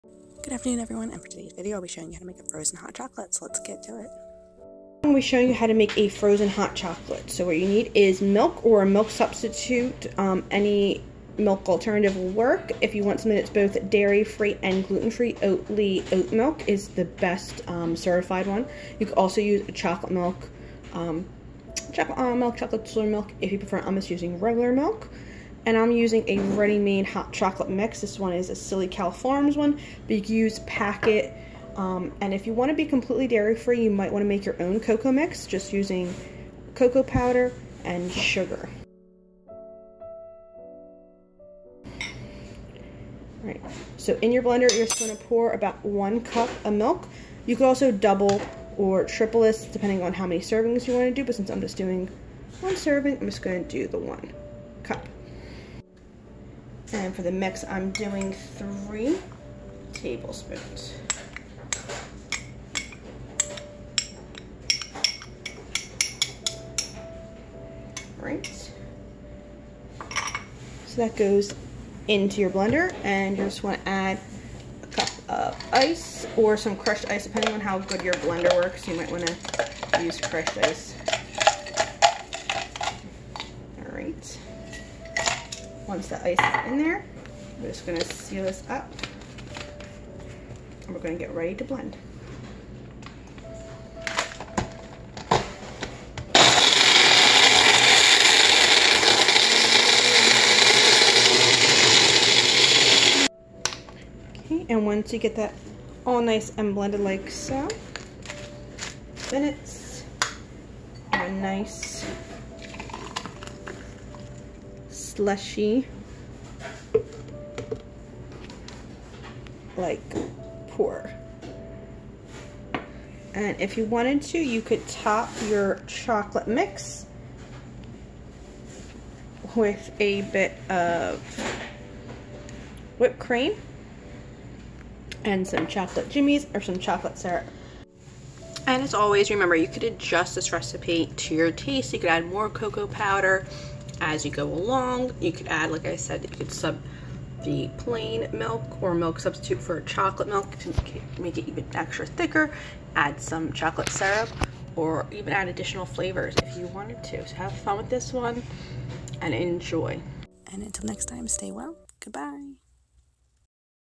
Recipes with Ms. Erica: Frozen Hot Chocolate
Is it Christmas in July??  Make some yummy frozen hot chocolate! From 7/5/2023